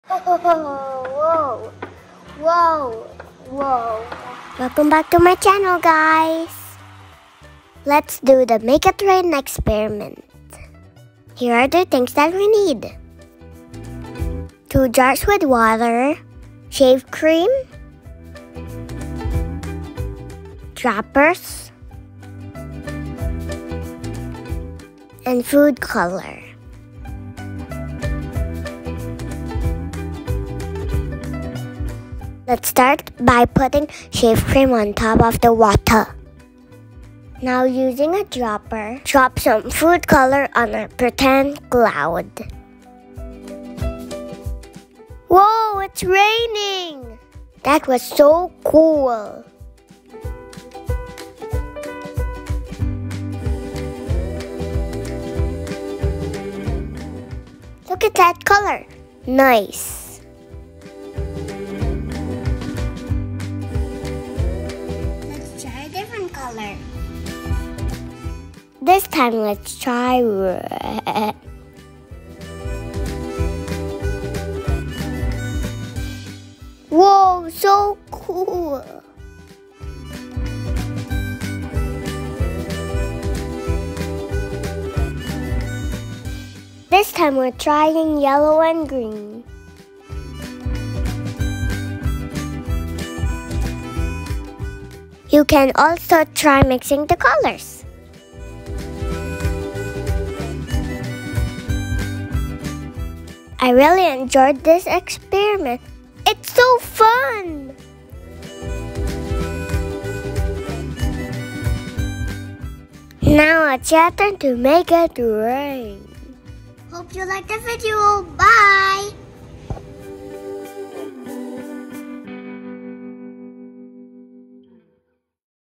Am-am’s “Make It Rain” Experiment
“Make it Rain” experiment inspired by @abcdeelearning7938

Quick, Easy and FUN!
Things we used:
Jars with water
Shave cream
Droppers
Food color